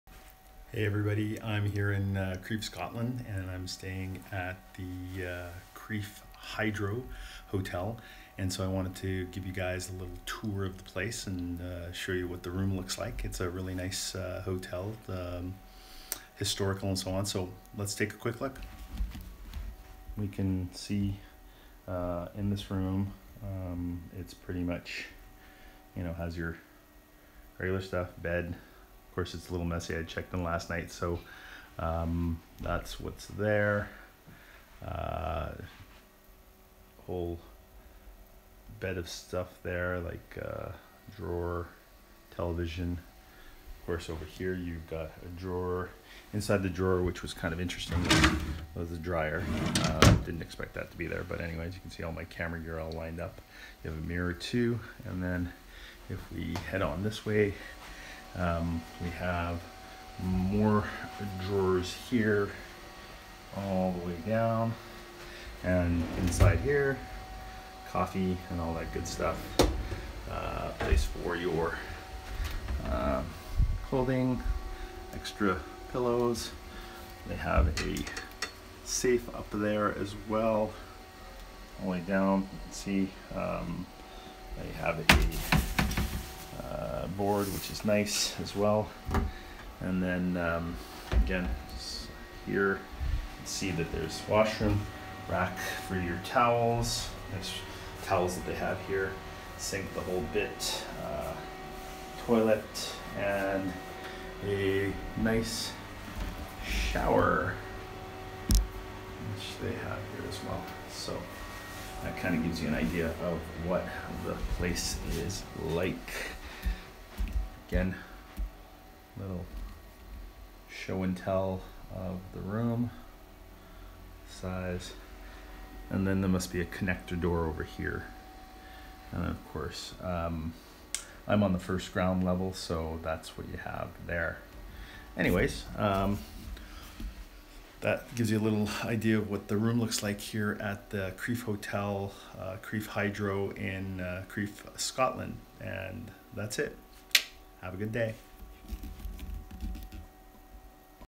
Crieff Hydro Hotel Scotland
A quick overview of a room t the Crieff Hydro in Scotland. Shot with Zhiyun Smooth Q and iPhone 7.